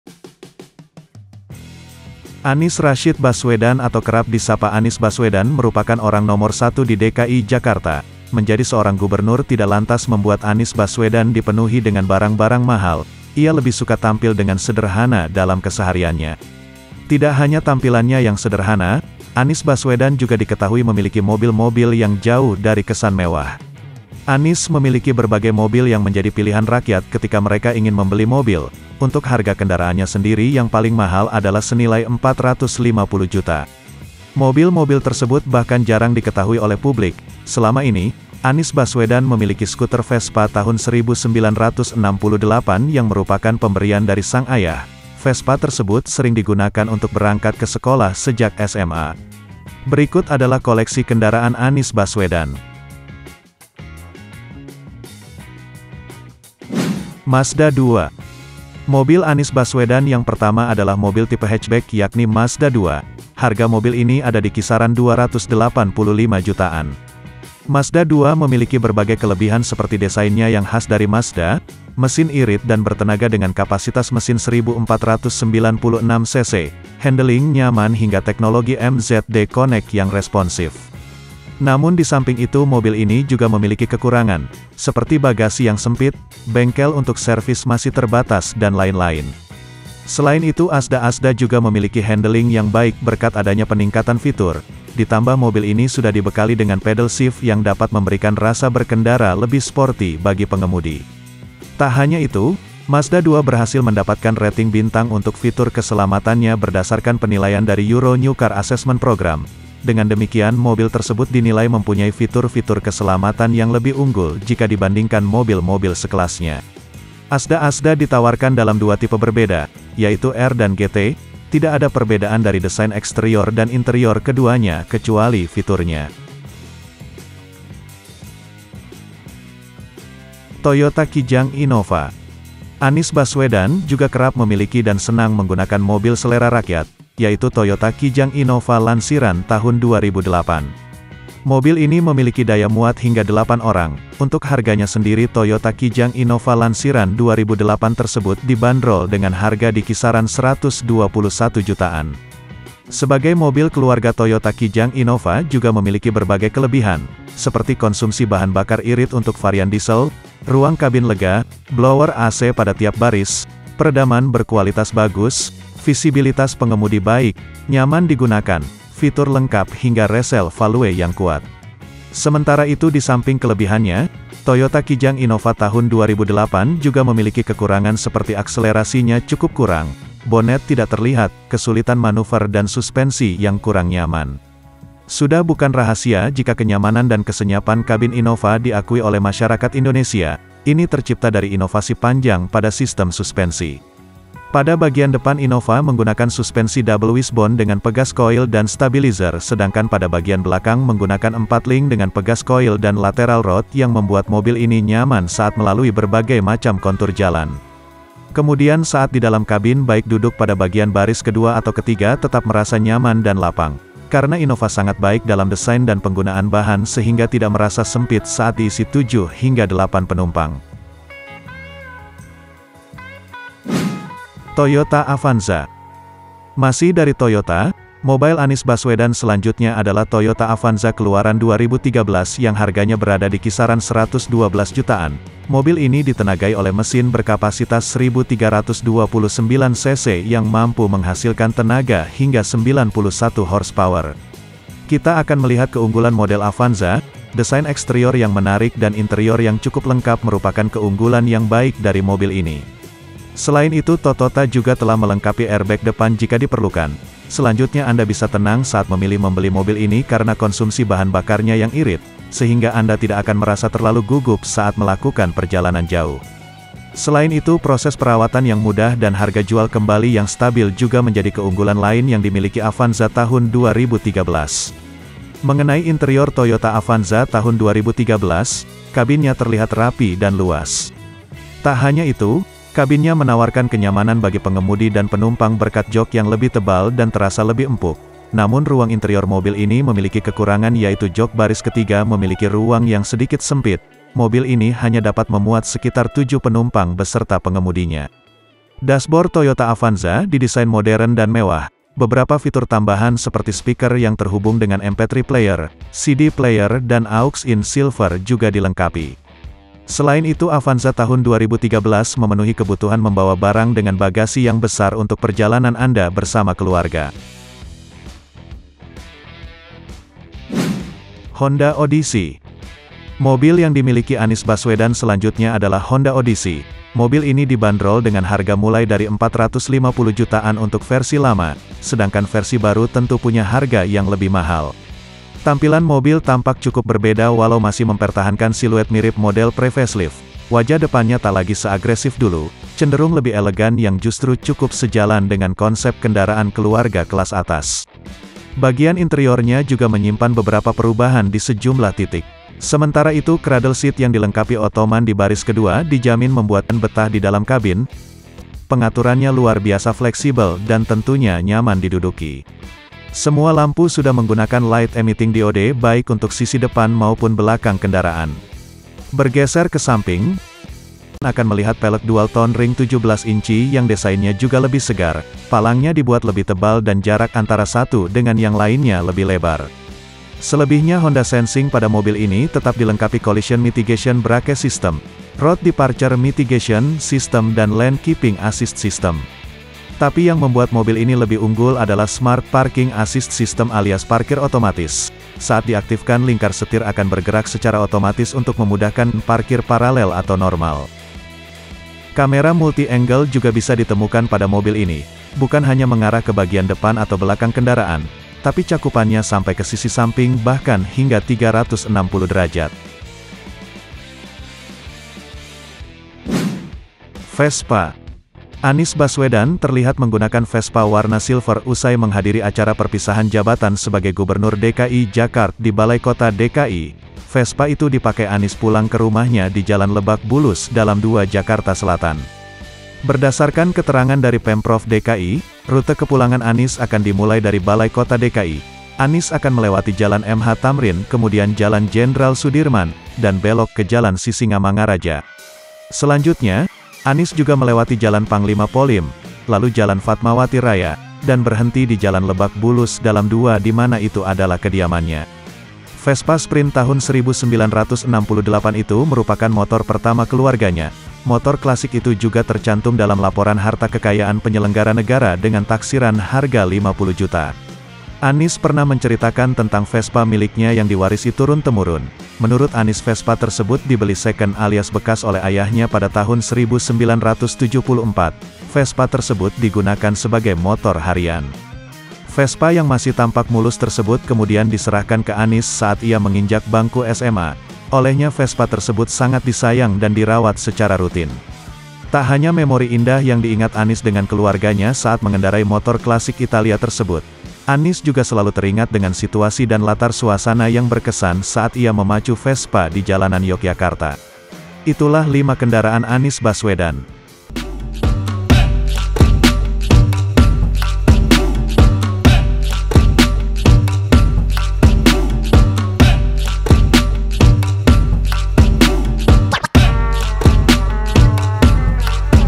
KOLEKSI KENDARAAN ANIES BASWEDAN YANG JAUH DARI KESAN MEWAH
Anies Baswedan adalah seorang gubernur Jakarta yang sederhana, mau tahu kendaraan apa saja yang dimiliki oleh gubernur Jakarta ini
tonton sampai habis video tentang koleksi kendaraan anies baswesan